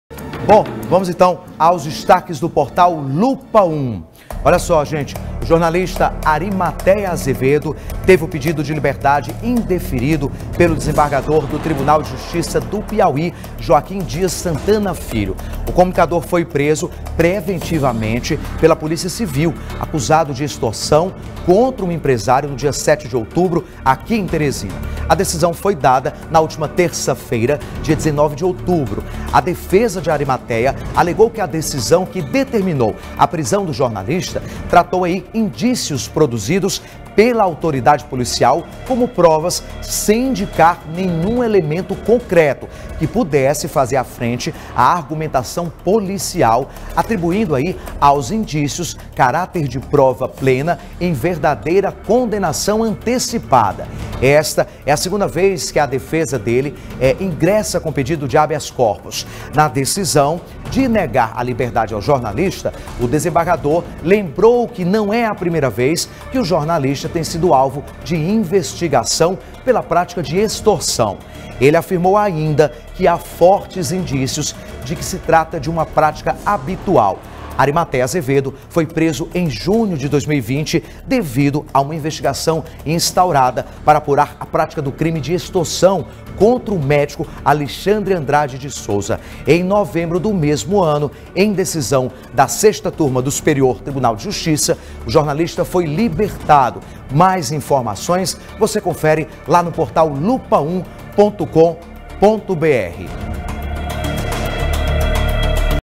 Desembargador nega pedido de liberdade do jornalista Arimatéia Azevedo
O comunicador foi preso no dia 7 de outubro deste ano acusado de extorsão.